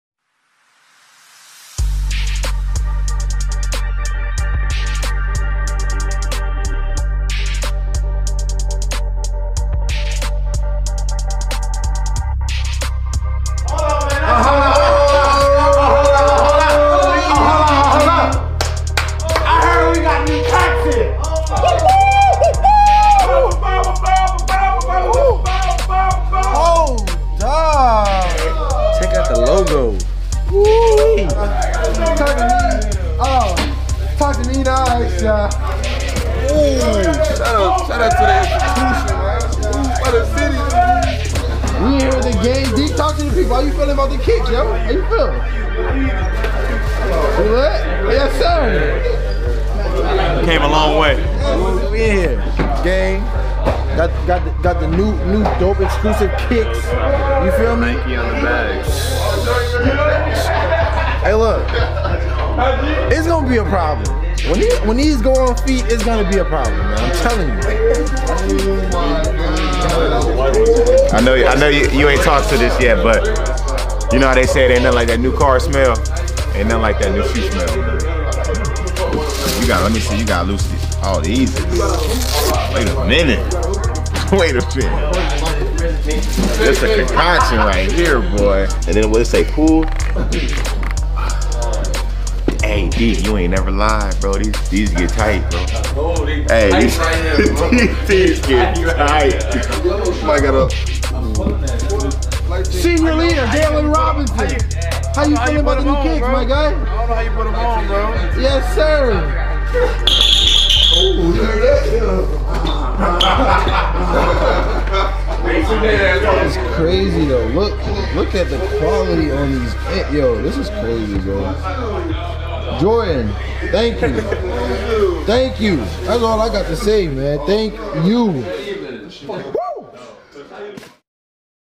University of Houston - "We are Jordan" - time for new shoes
Coogs received their @UHouston Jordan Brand shoes this afternoon in Guy V Lewis Development Facility locker room

They seemed to like them!